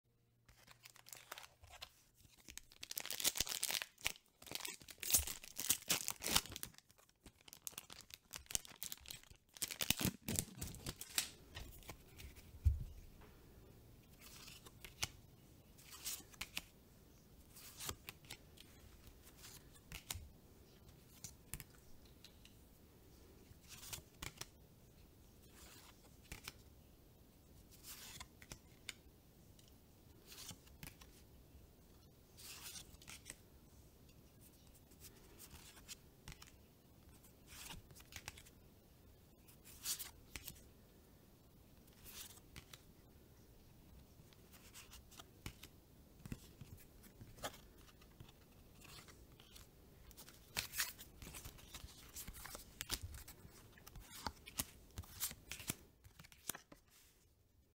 Wednesday Night D&D!! - Commander Legends Battle for Baldur's Gate - Collector Booster
Dungeons & Dragons storytelling meets Magic’s most social format in a new crossover multiplayer experience for the ages. Turn every Commander game into an adventure with iconic D&D characters and spells. And create your own powerful character from the ground up with the return of Commander Draft.